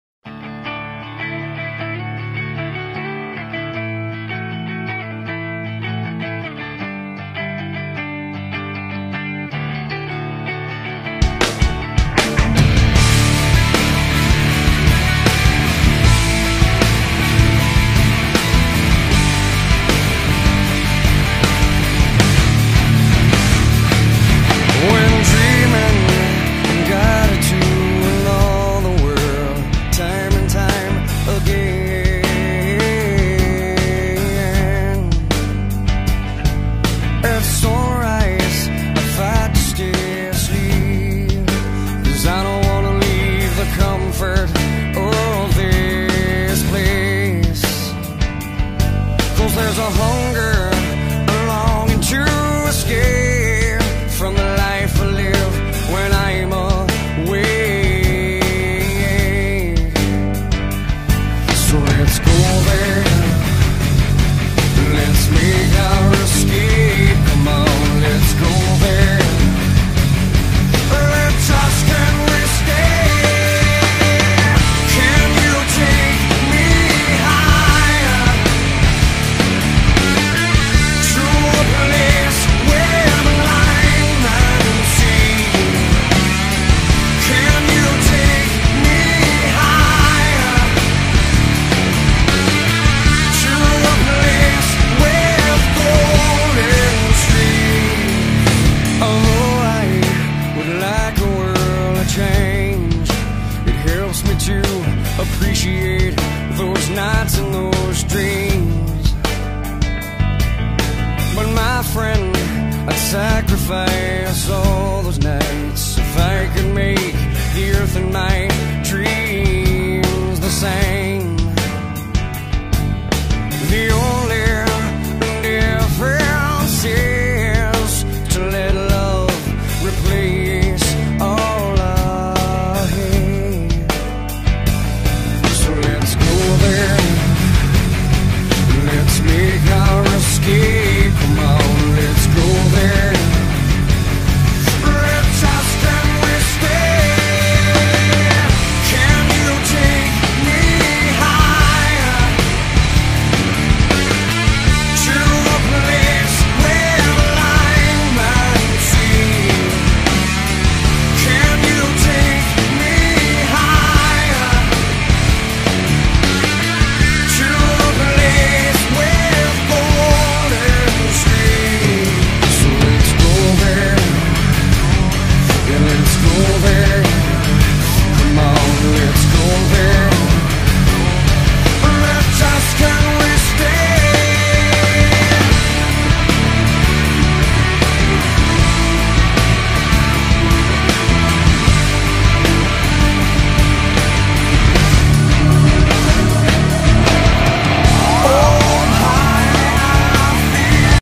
mighty bulldog
American bulldogs on training.